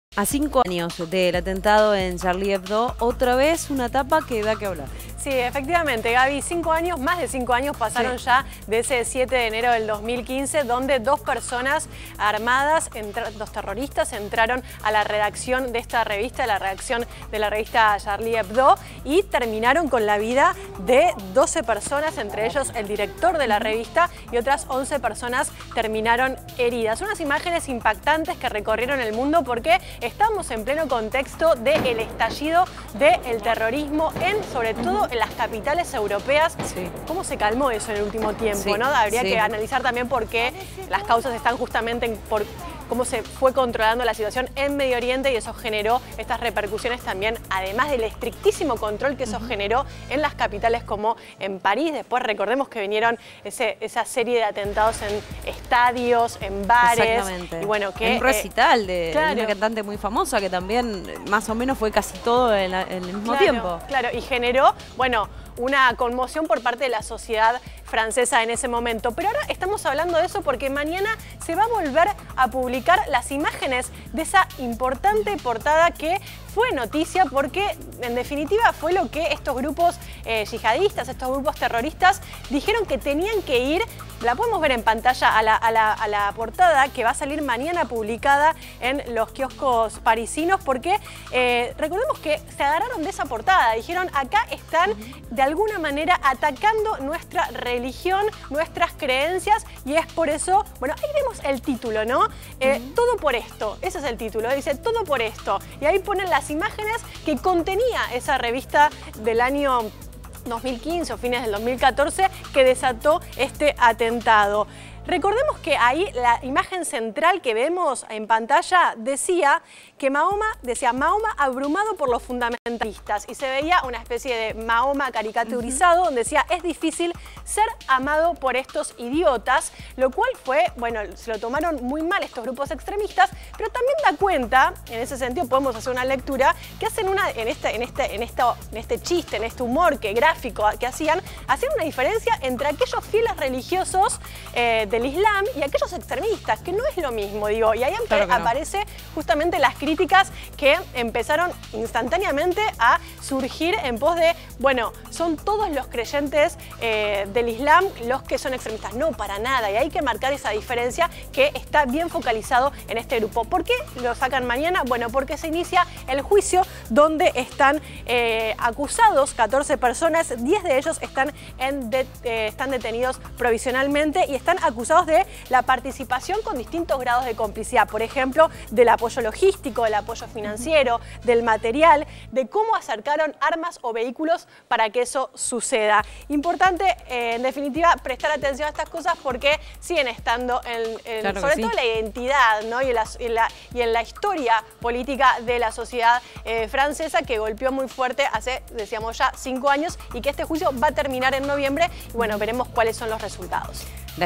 A 5 años del atentado en Charlie Hebdo
Arranca el juicio 5 años después y la revista vuelve a publicar la viñeta de Mahoma. El 7 de enero de 2015, los hermanos Saïd y Chérif Kouachi, entraron disparando armas automáticas en la redacción de la revista parisina matando a 12 personas.